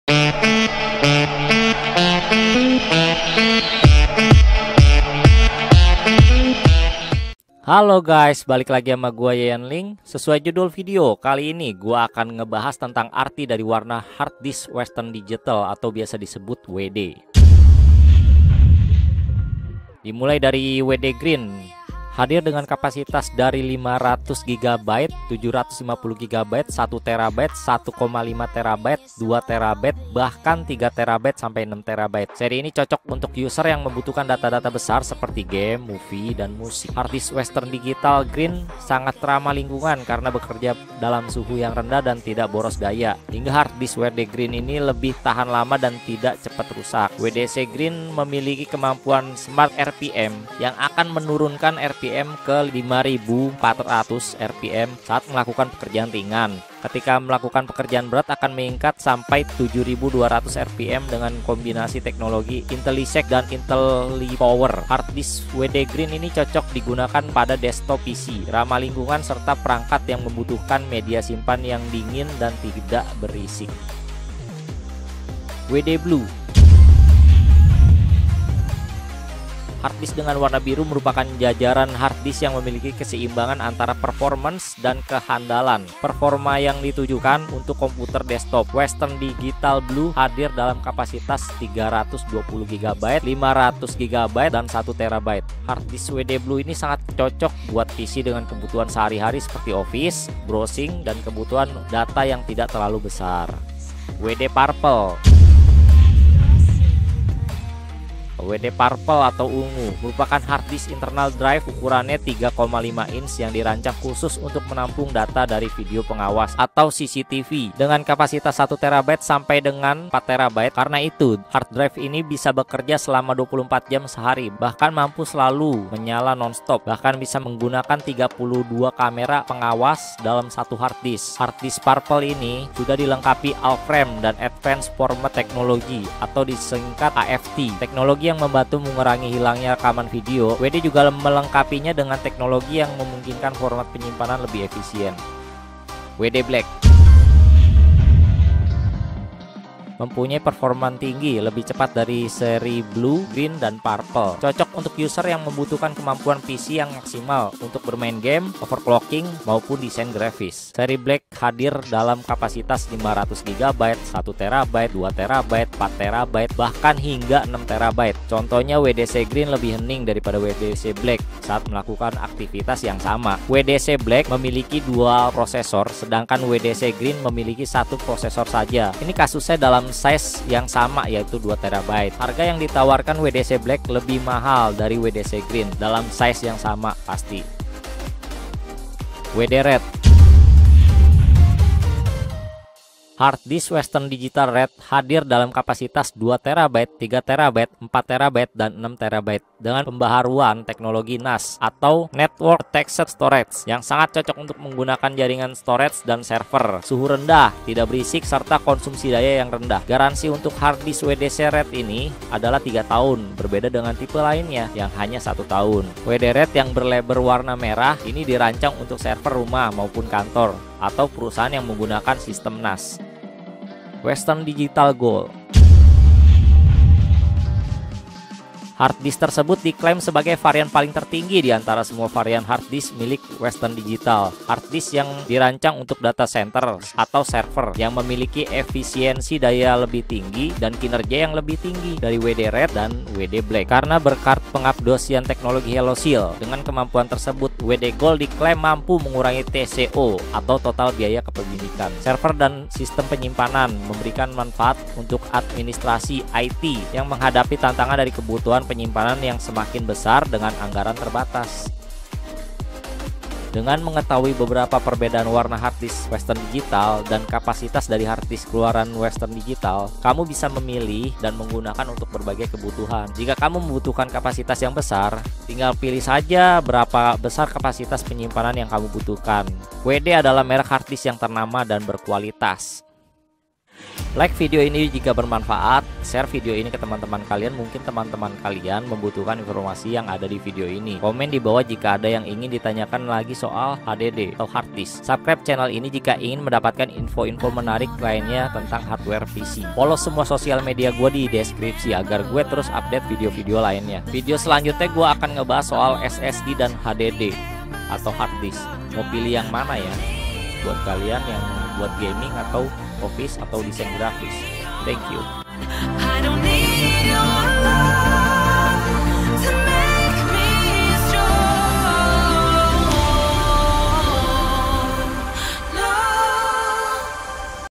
Perbedaan Warna Hardisk WD Green,Blue,Purple,Black, Red dan Gold
Arti warna HDD WD Green,Blue,Purple,Black, Red dan Gold
Sangat penting untuk artinya,bukan cuma variasi doang :D

Budayakan Like sebelum nonton
Share video ini ke teman-teman kalian
Commant jika ada pertanyaan atau ada yg ingin di sampaikan